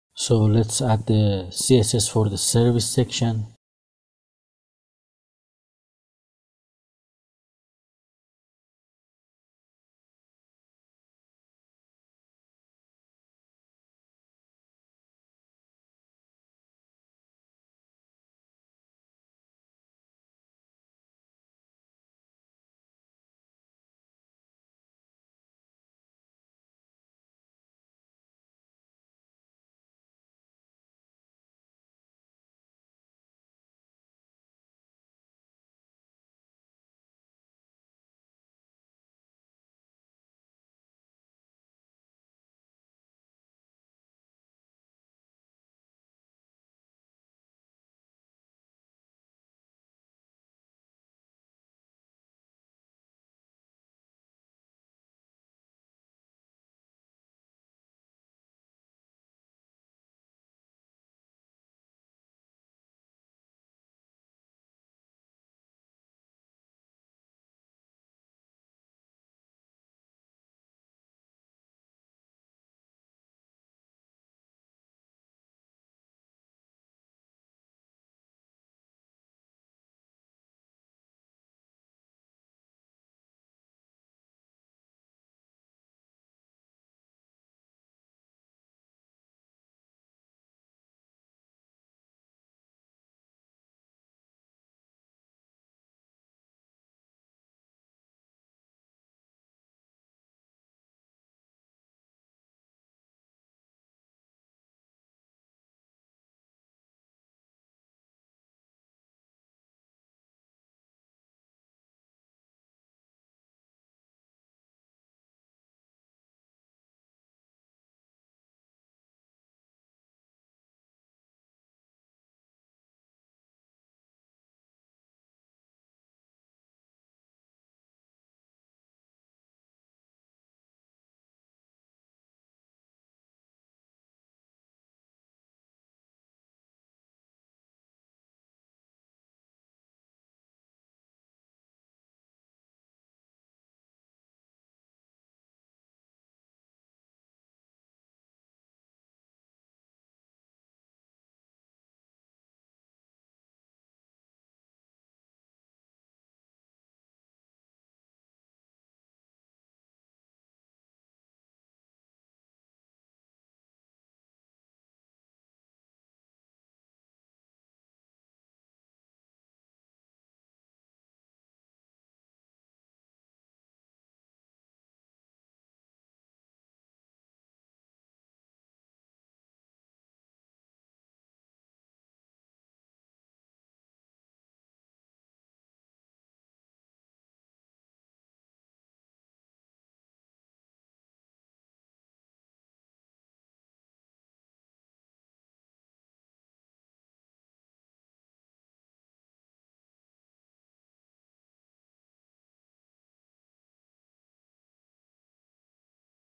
Build Responsive Website using HTML5 & CSS3 From Scratch / Part 12
Build Responsive Website using HTML5 & CSS3 From Scratch /
CSS - Style "service"